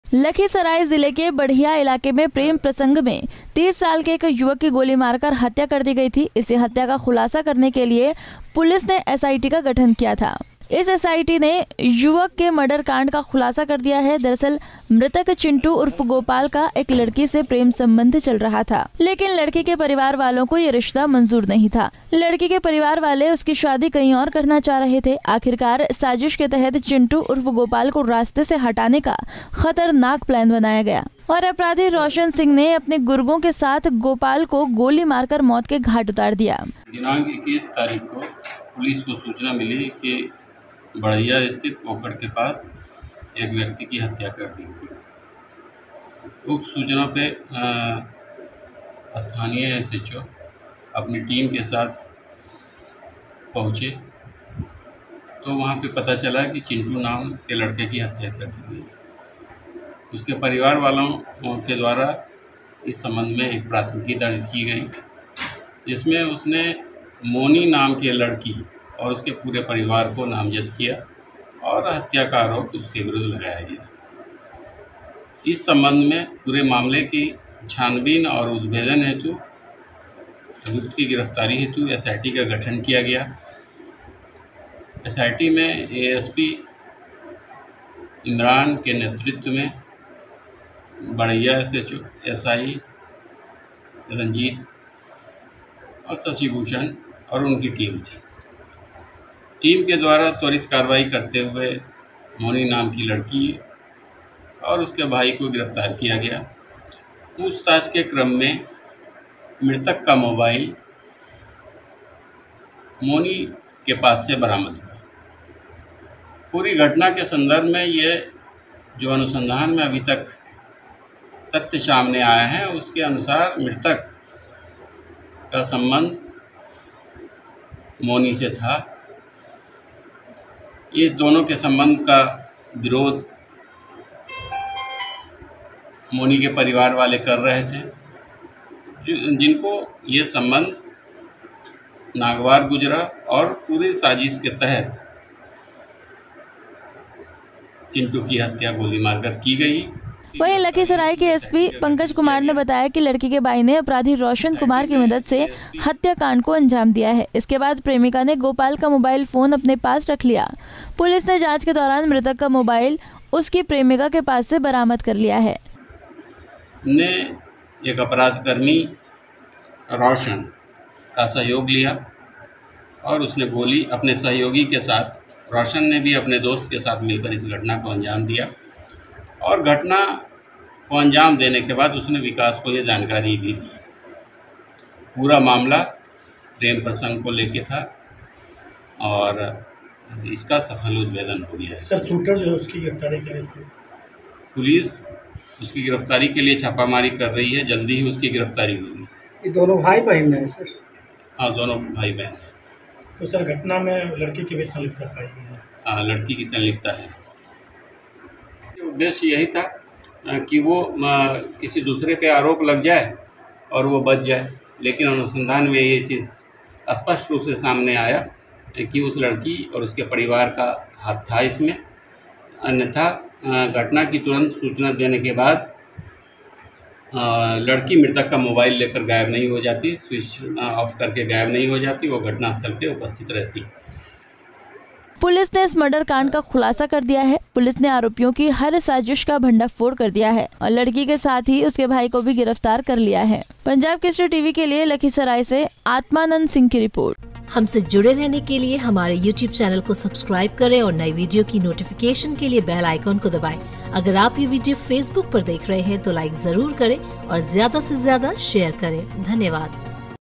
बेटी का लड़के से प्यार का रिश्ता स्वीकार नहीं कर पाए परिजन
बेटी का लड़के से प्यार का रिश्ता स्वीकार नहीं कर पाए परिजन, प्रेमी लड़के को गोली मार कर उतार दिया गया मौत के घाट

A 30-year-old man was shot dead in Lakhisarai by a woman's relatives as the family could not accept their relationship.